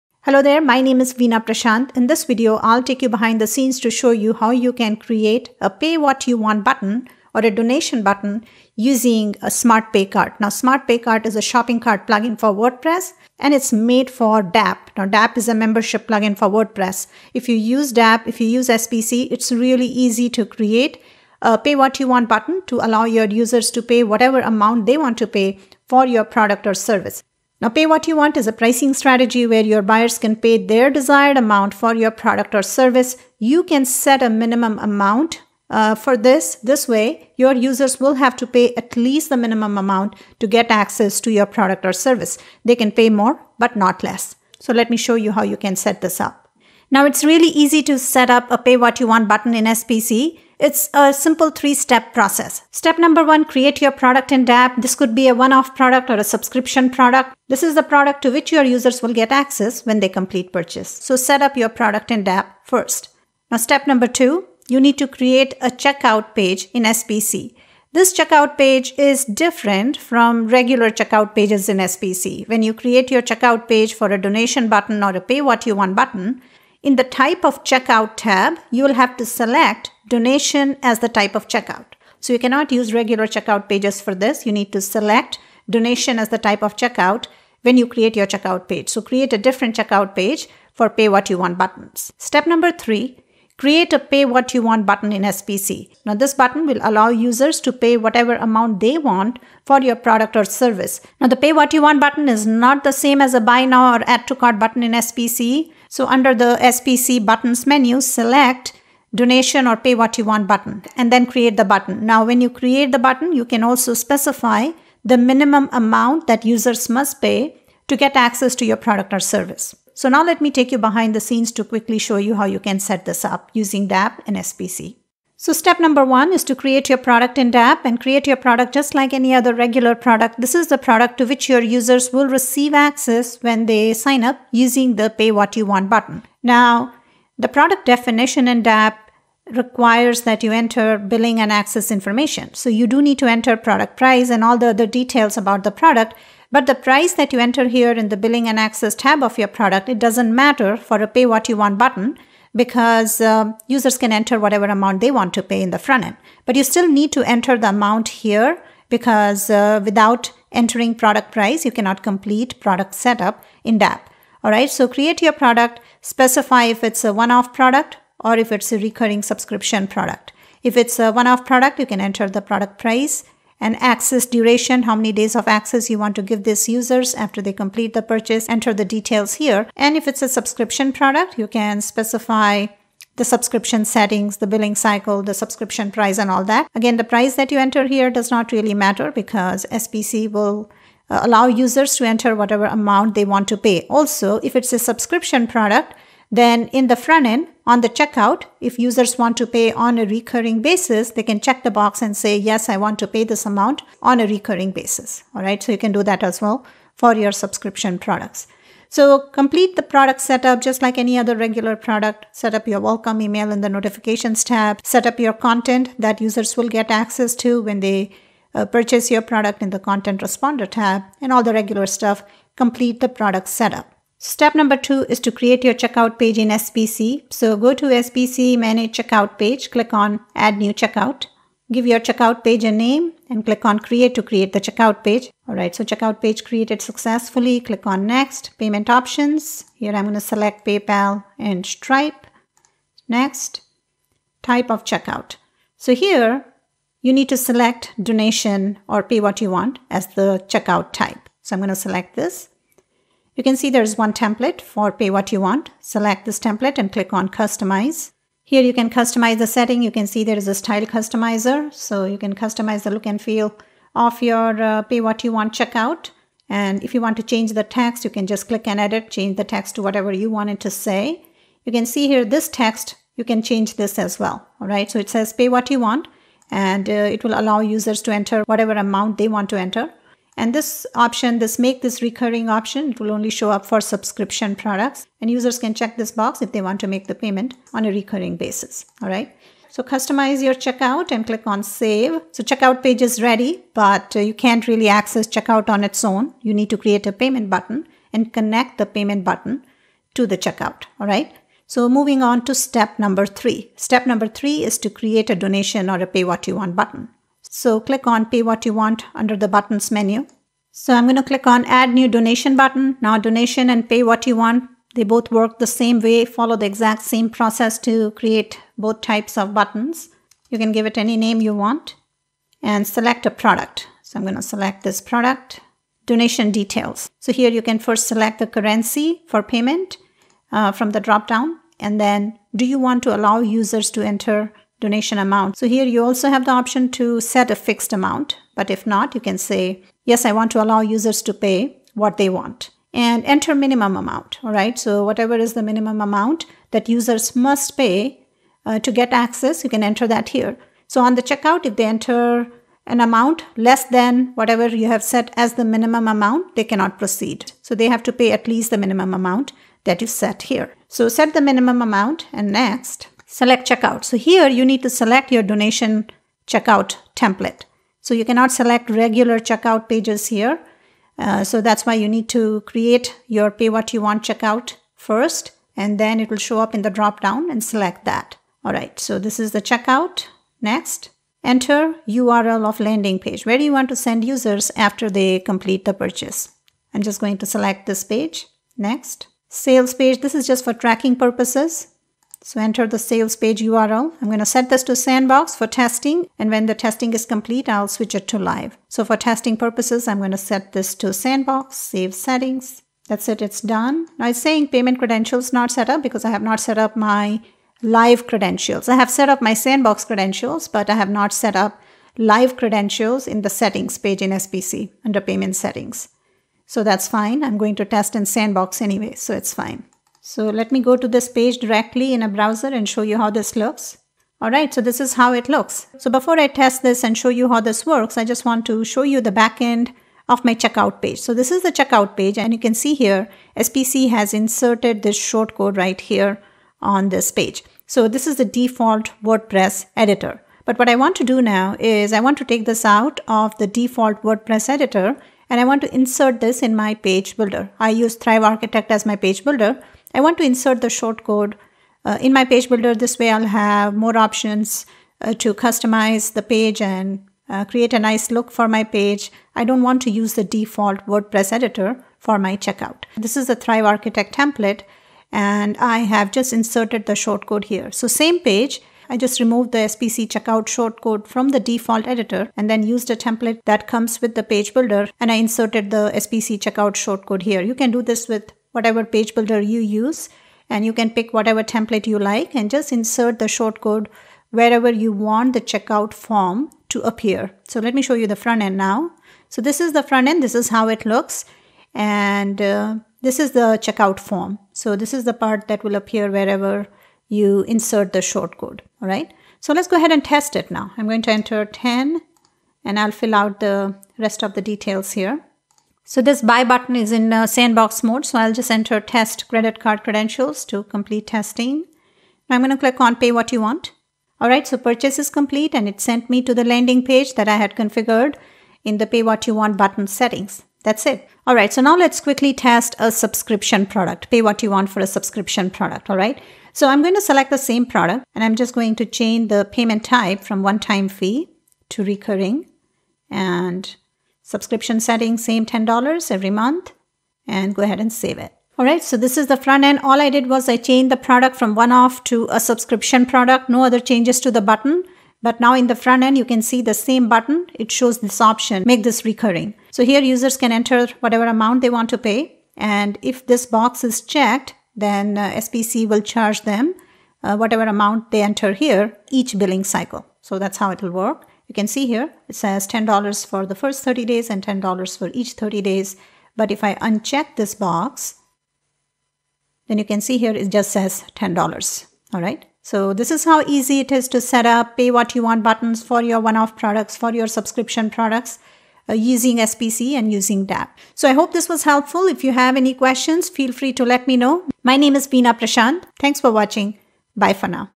Pay what you want (PWYW) /Donation Button!
Pay what you want (PWYW) is a pricing strategy where buyers pay their desired amount for your product or ser. And it's very to setup a PWYW button using SmartPayCart.com (SPC).

You can set a minimum amount for your PWYW buttons. This way users will have to pay at least the minimum amount to get access. They can pay more but not less than the minimum amount. Setting up PWYW buttons is a simple-3 step process in SPC. Please watch video to how to set it up in just a few minutes.

Membership Plugin: DigitalAccessPass.com
Shopping Cart Plugin: SmartPayCart.com